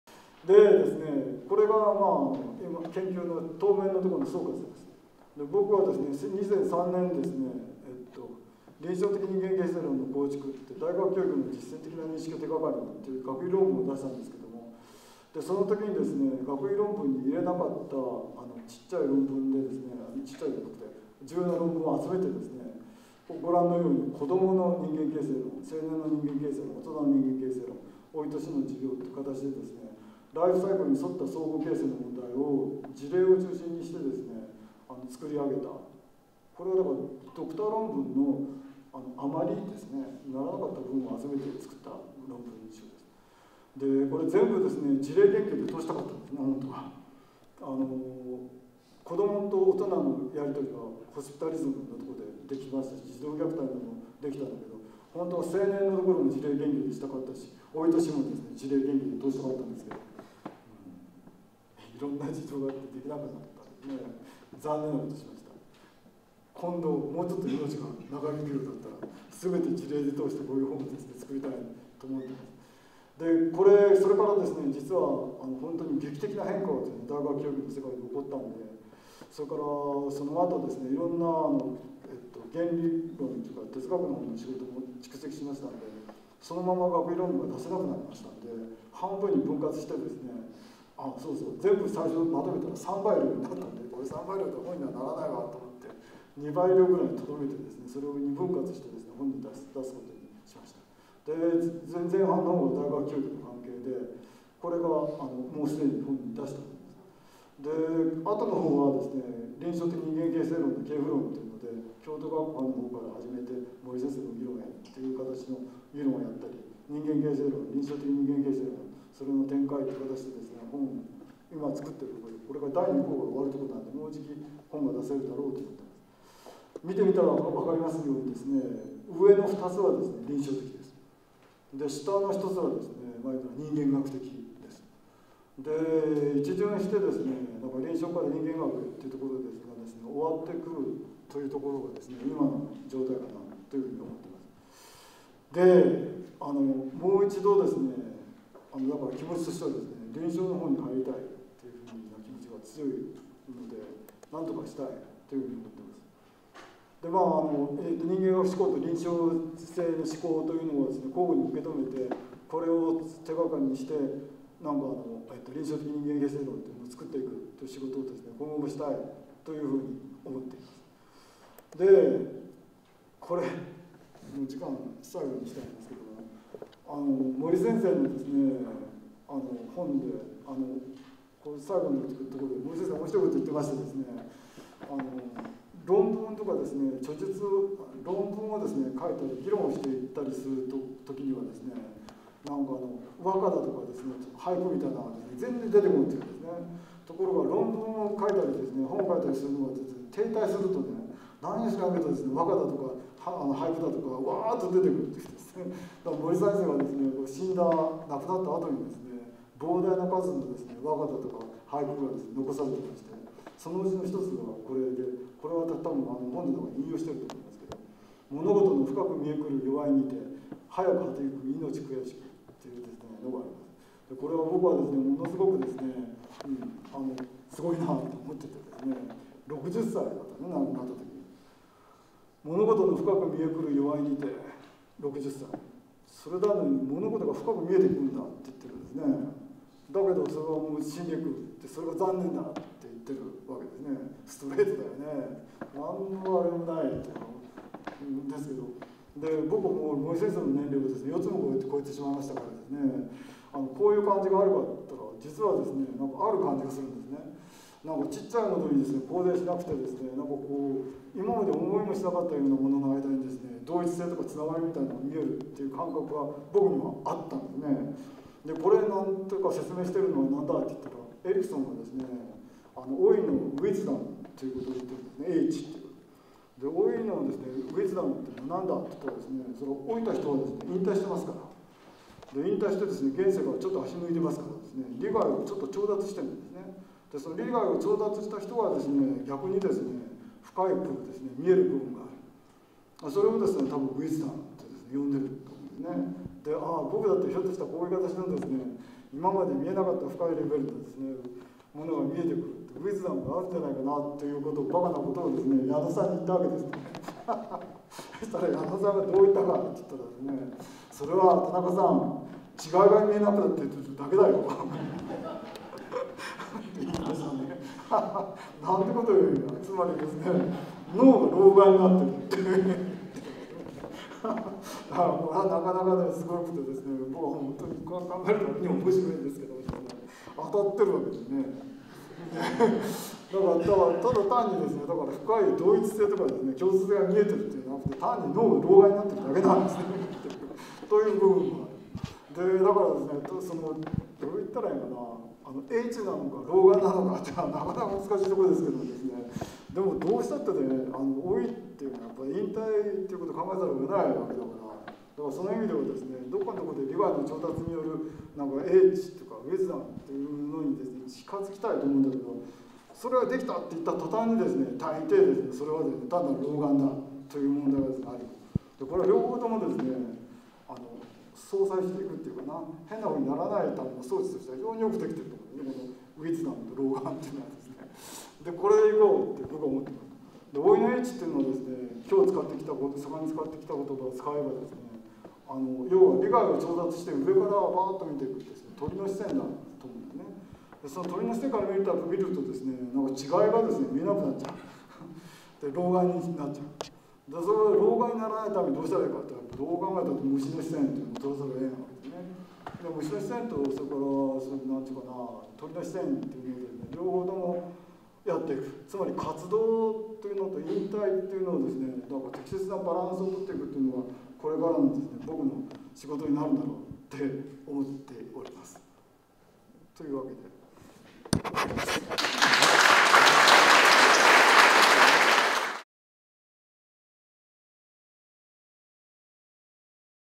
京都大学 田中 毎実 高等教育研究開発推進センター教授 最終講義-08
京都大学
田中毎実 高等教育研究開発推進センター教授 最終講義
「臨床的人間形成論へ -これまでとこれから-」
2012年3月17日（土）
京都大学楽友会館 2階 会議・講演室

こちらで一連の講義が視聴できます。